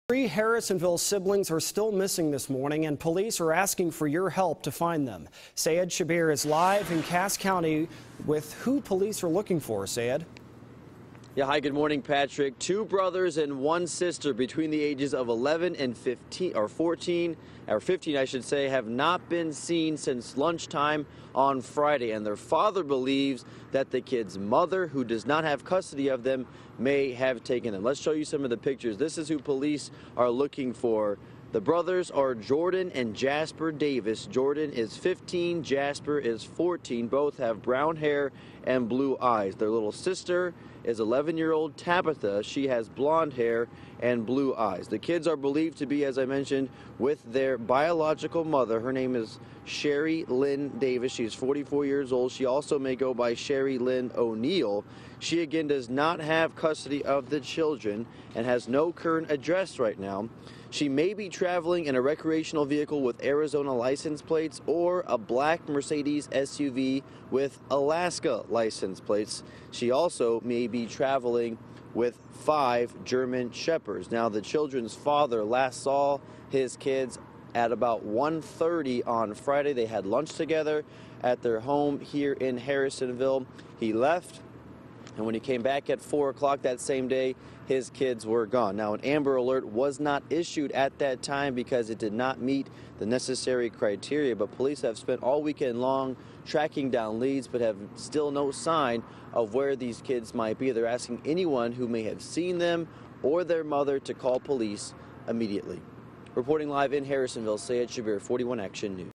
Three Harrisonville siblings missing
he Cass County Sheriff has issued a Missing Persons Alert for three young siblings last seen at their Harrisonville, Mo., home Friday afternoon, Oct. 11.